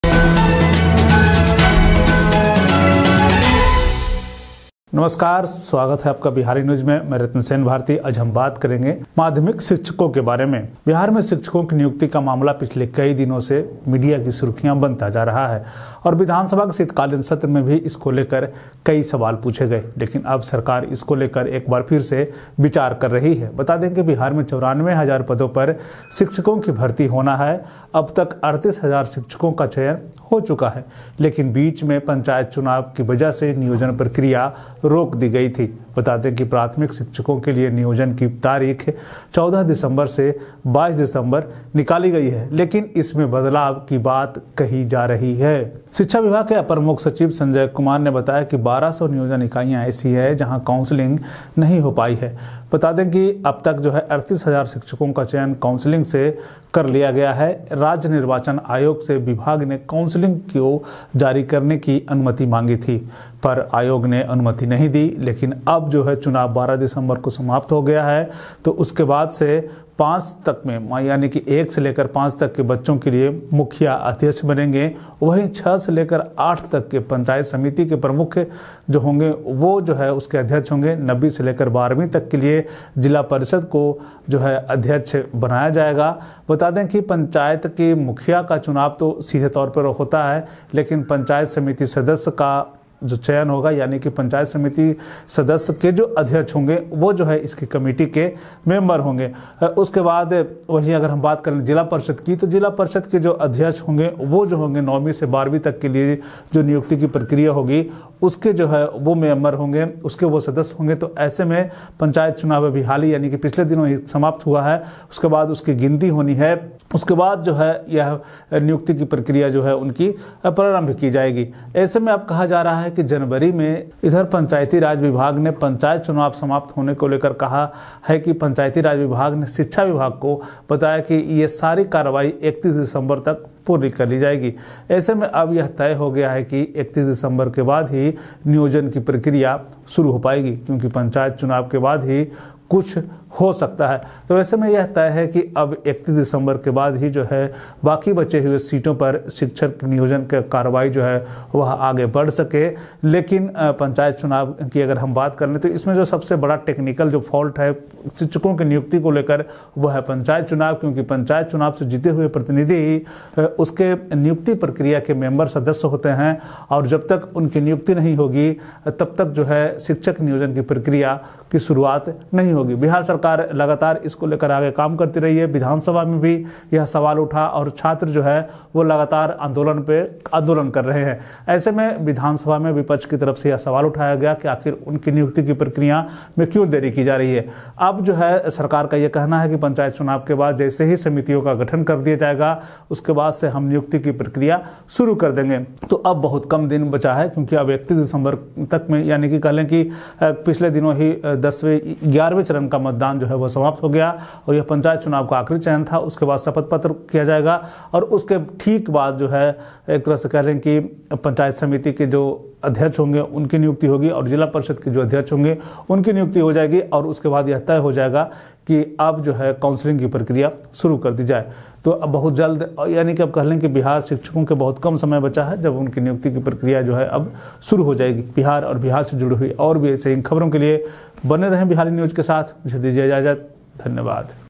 जनवरी में होगी माध्यमिक शिक्षकों की काउंसलिंग, इसलिए हो रही है देरी
The issue of appointment of teachers in Bihar has been making media headlines for the last several days and the question was asked in the winter session of the Vidhan Sabha as well, but now the government is considering it once again. Let us inform that in Bihar, teachers are to be recruited for 94 thousand posts. So far 38 thousand teachers have been selected. But due to the Panchayat elections in between, the planning process has been stopped.